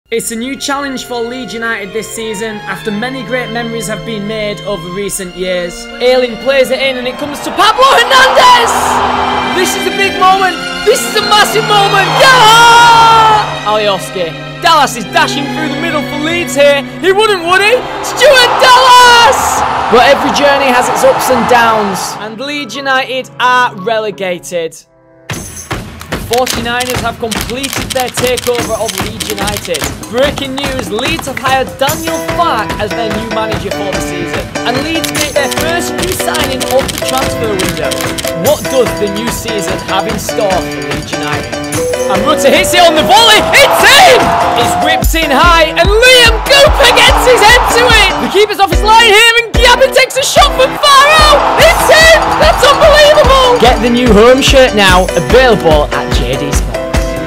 Will Leeds United be PROMOTED this season?⚽
What will the 2023/2024 season hold for Leeds United. Many great memories have been made over recent years for Leeds such as Pablo Hernandez scoring against Swansea and Dallas scoring a last minute winner against Manchester City. A new challenge for Leeds United with new owners and new signings